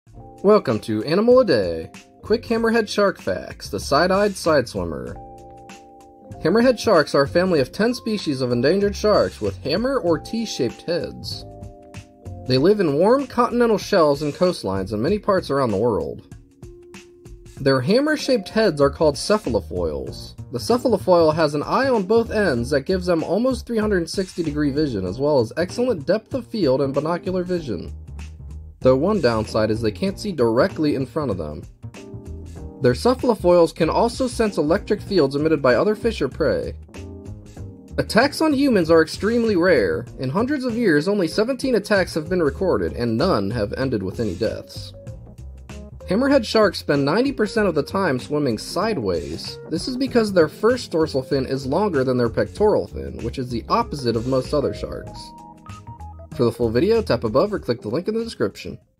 Quick Hammerhead Shark Facts - The Side-Eyed Side-Swimmer - Animal a Day
Today we learn about the nearly harmless shark with a hammer for a head, the Hammerhead Shark.